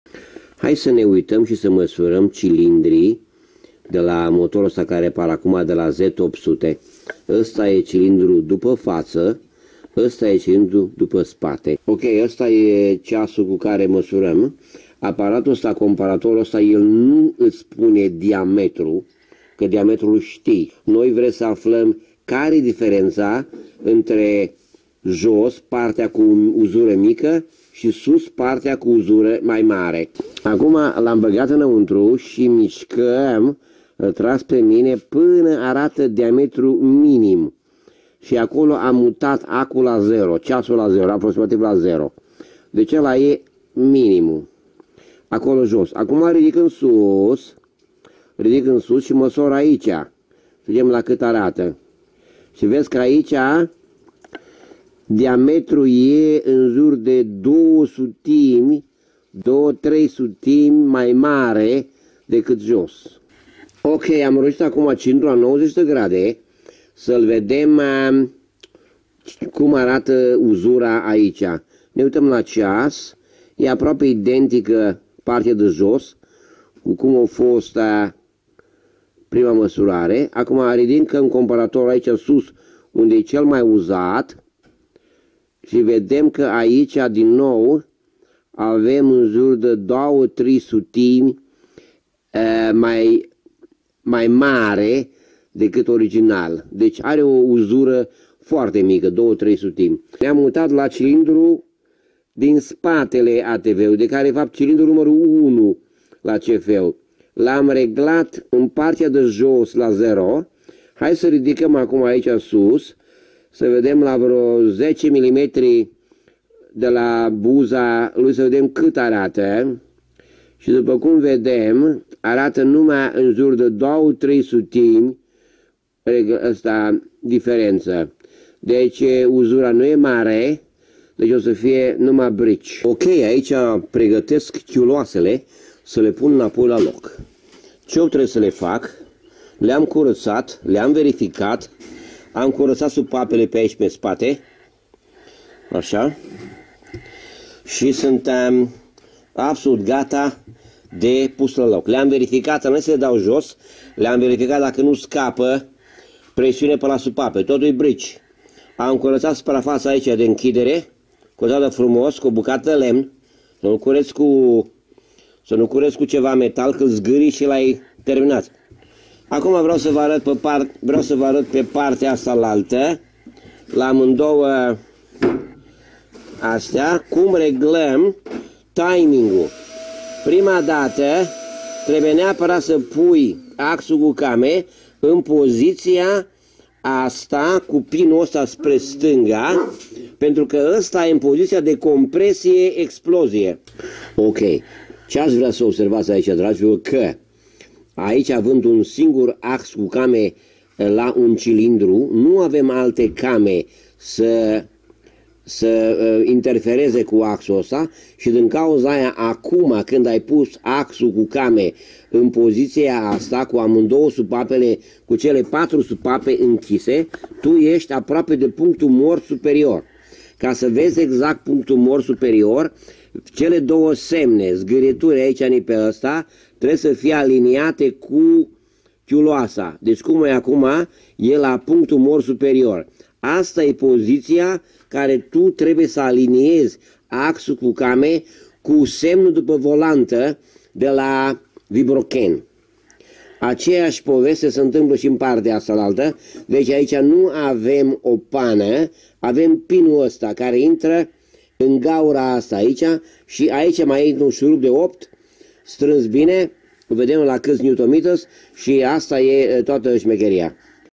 How I Check cylinder wear with a dial gouge for a CFmoto 800
CF Moto Z 800 cc engine , i check cylinders  for wear , and get  head cylinders ready to be fitted